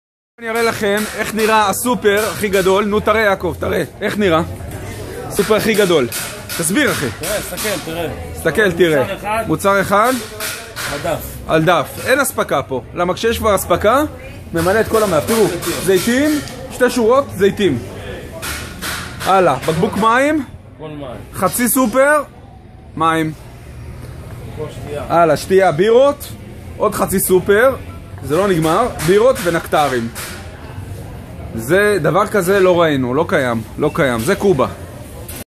super cuba
a tour in a common super market in Havana Cuba